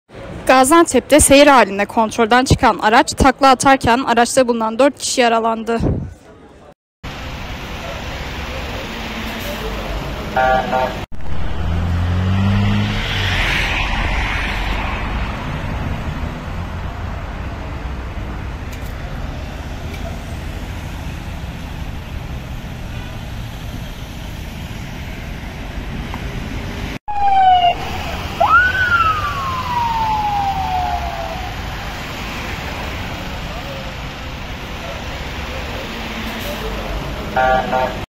Takla Atan Araçta Bulunan 4 Kişi Yaralandı
Gaziantep’te seyir halinde kontrolden çıkan araç takla atarken, araçta bulunan 4 kişi yaralandı